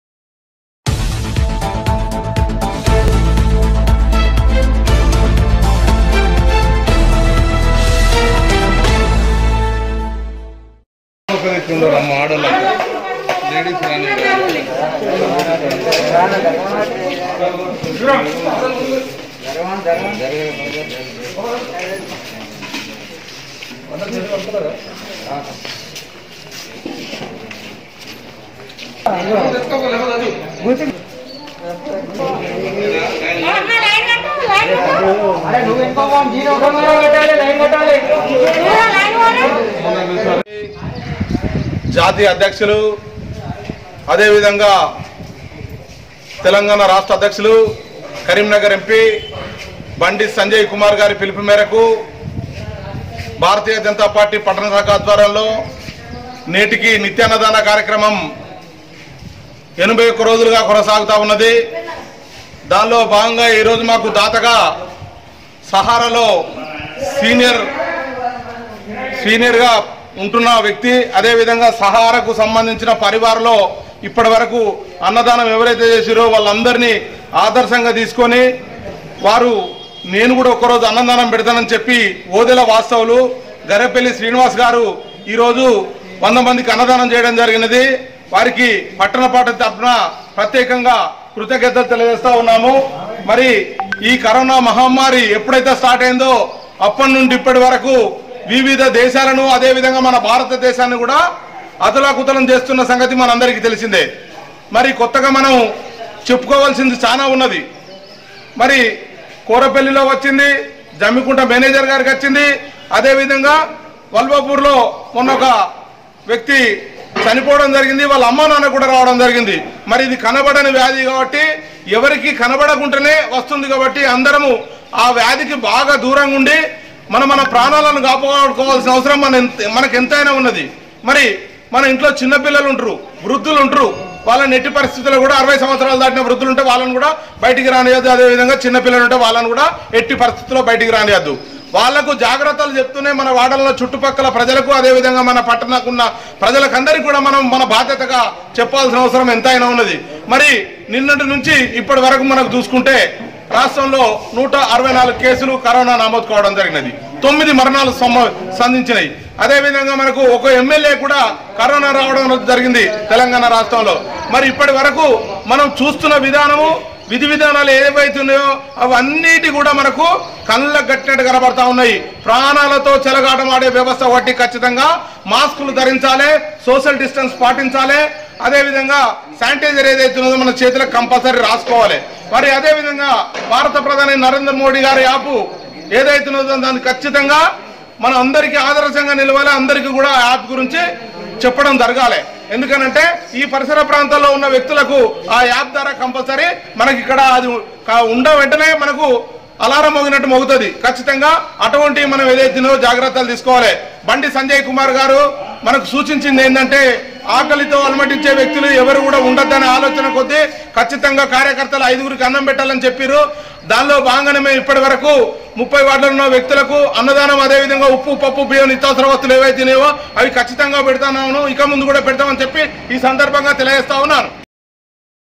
2039 జీడి మల్లేష్ ఆధ్వర్యంలో ఓదెల వాస్తవ్యులు గర్రెపల్లి శ్రీనివాస్ 100 మందికి అన్నదానం ..
జీడి మల్లేష్ ఆధ్వర్యంలో ఓదెల వాస్తవ్యులు గర్రెపల్లి శ్రీనివాస్ 100 మందికి అన్నదానం ..